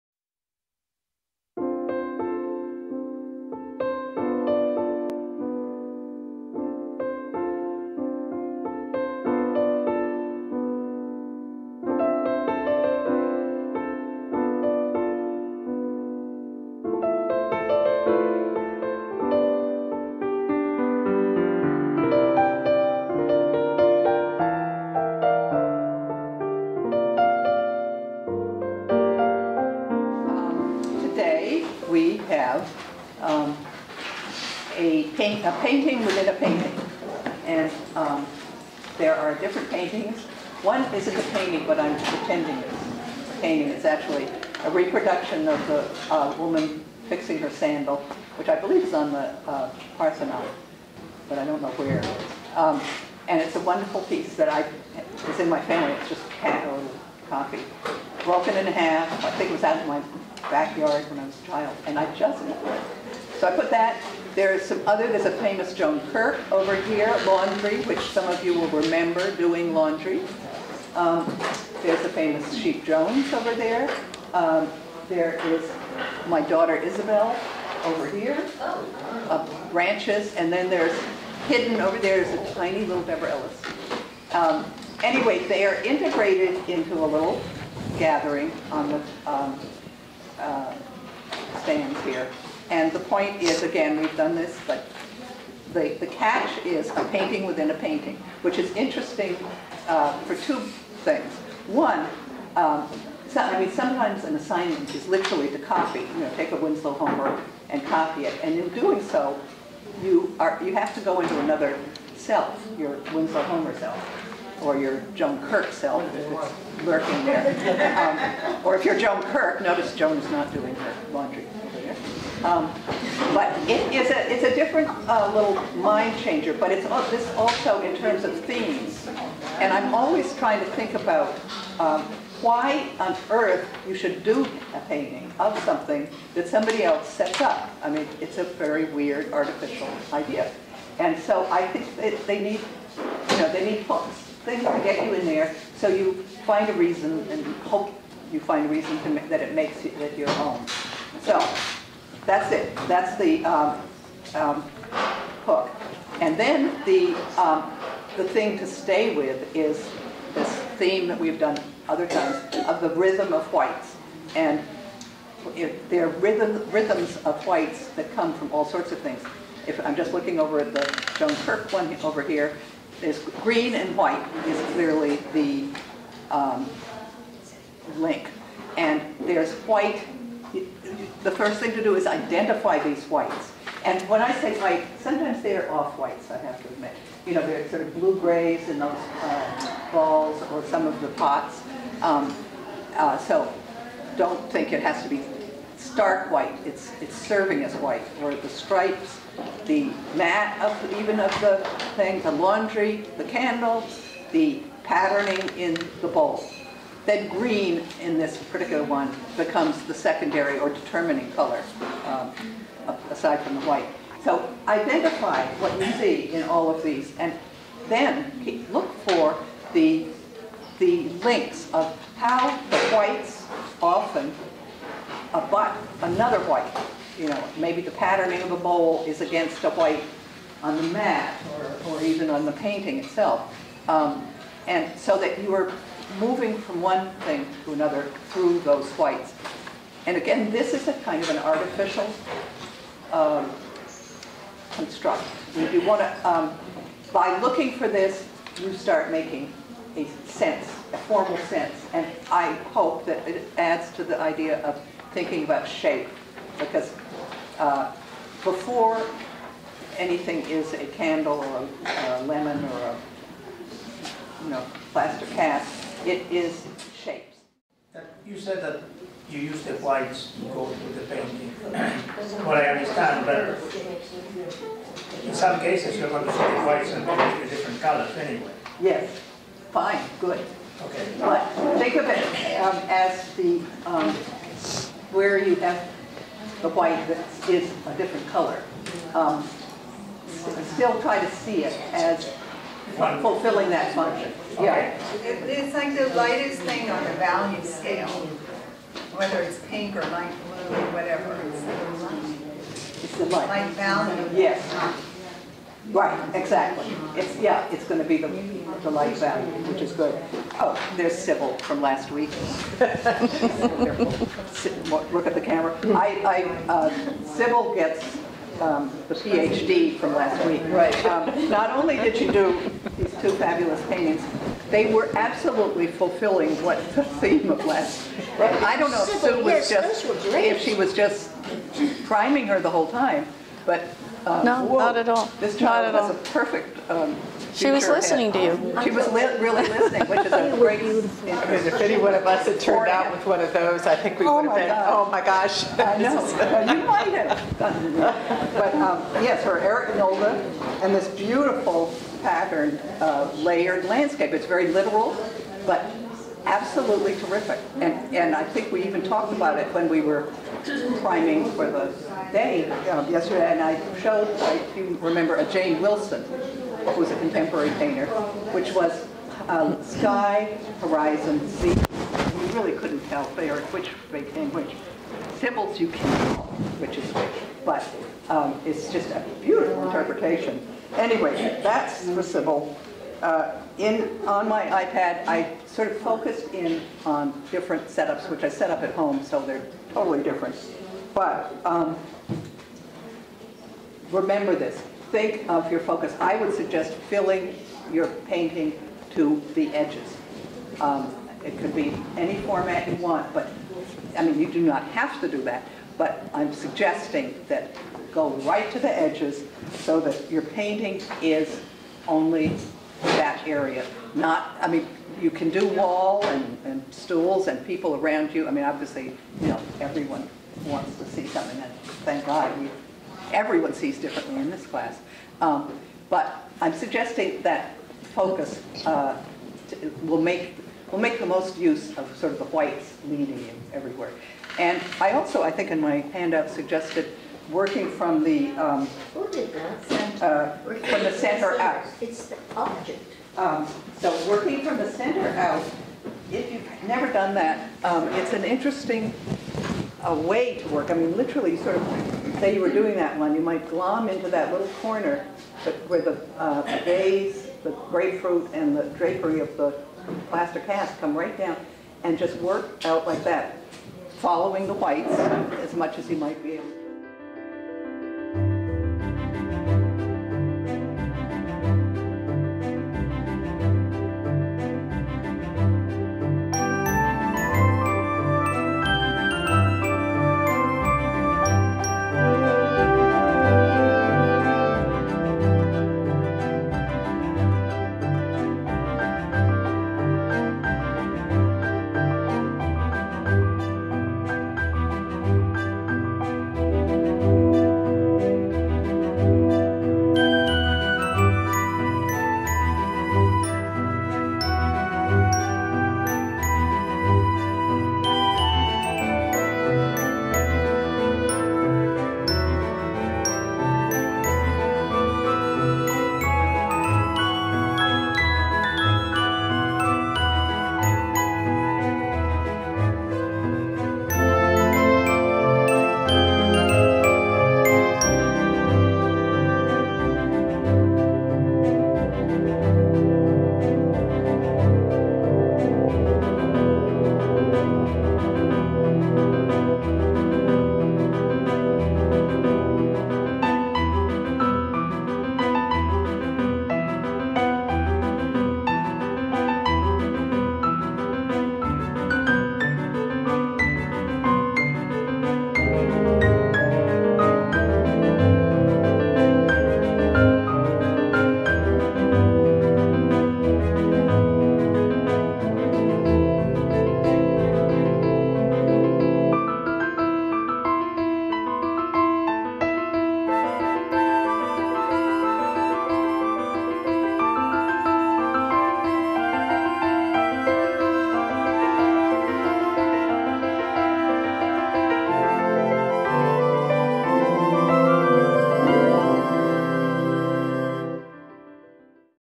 Deborah Ellis Watercolor Assignment: Rhythm of Whites
Deborah Ellis talks about watercolor -- the search for rhythm of whites and paintings within a painting.